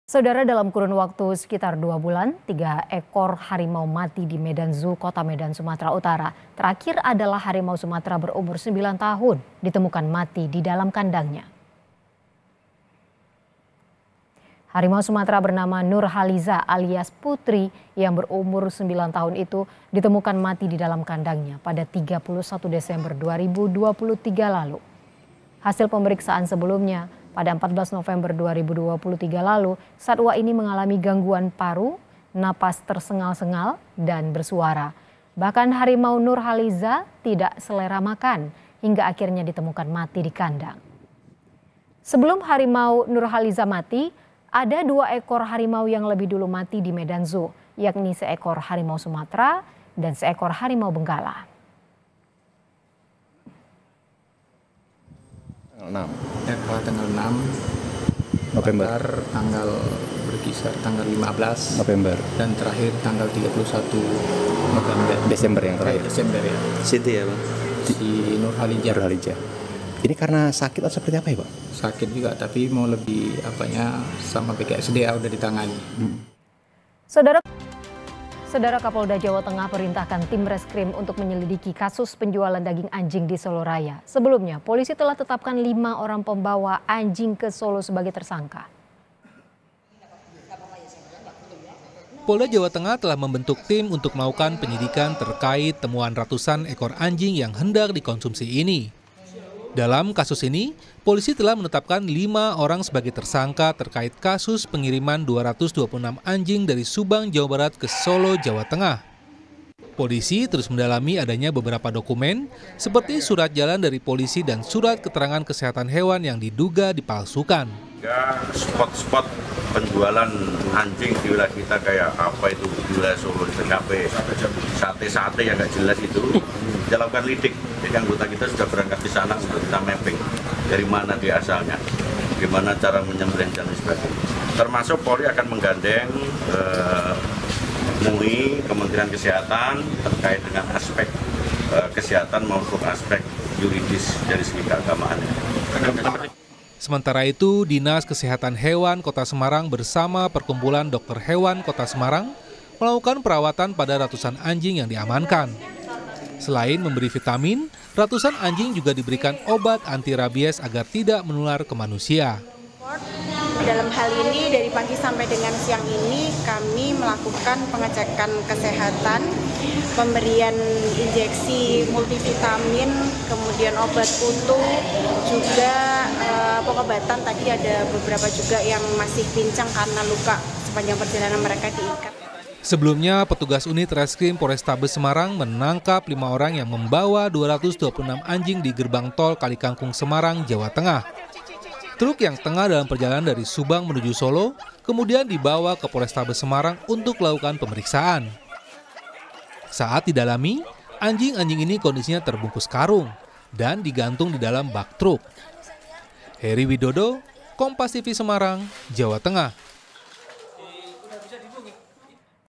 Krisis Medan Zoo, Tiga Harimau Mati dalam Waktu 2 Bulan
MEDAN, KOMPAS.TV - Dalam kurun waktu sekitar 2 bulan 3 ekor harimau mati di Medan Zoo, Kota Medan, Sumatera Utara. Terakhir adalah harimau sumatera berumur 9 tahun ditemukan mati di dalam kandangnya.

Harimau Sumatera bernama Nurhaliza alias Putri yang berumur 9 tahun itu ditemukan mati di dalam kandangnya pada 31 Desember 2023 lalu.

Hasil pemeriksaan sebelumnya pada 14 November 2023 lalu, satwa ini mengalami gangguan paru napas tersengal-sengal dan bersuara. Bahkan harimau Nurhalizah tidak selera makan hingga akhirnya ditemukan mati di kendang.

Sebelum harimau Nurhaliza mati ada 2 ekor harimau yang lebih dulu mati di Medan Zoo yakni seekor harimau Sumatera dan seekor harimau benggala.